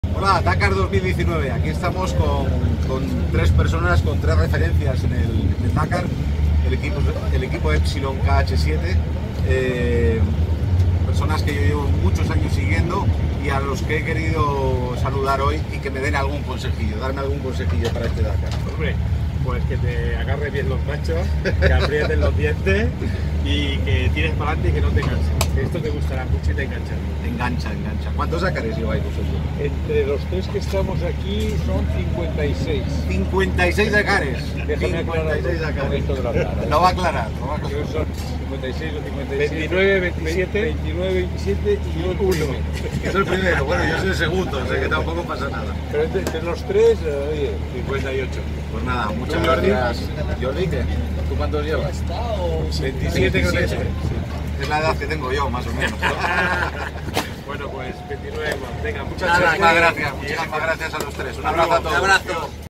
Rally Dakar 2019 - Palibex junto a los veteranos del KH-7 Epsilon Team
Agarrarse los machos, apretar los dientes y tirar para adelante. Sabios consejos los que hemos recibido de los integrantes del KH-7 EPSILON TEAM, uno de los equipos más veteranos del Rally Dakar.

El equipo, uno de los históricos del Dakar, está liderado por Jordi Juvanteny y José Luis Criado, una de las parejas más populares de esta prueba ya que suman entre ambos más de 50 participaciones.

Junto a ellos, la tripulación se verá completada por Xavi Domènech, debutante en esta edición.

¡Gas a fondo!

Grab the site, grit your teeth and pull forward. Wise advices received from the members of the KH-7 EPSILON TEAM, one of the most veteran teams of the Dakar Rally.

The team, one of the historic teams of the Dakar, is led by Jordi Juvanteny and José Luis Criado, one of the most popular couples of this event since they have more than 50 participations between them.

Along with them, the crew will be completed by Xavi Domènech, debutant in this edition.

Full power!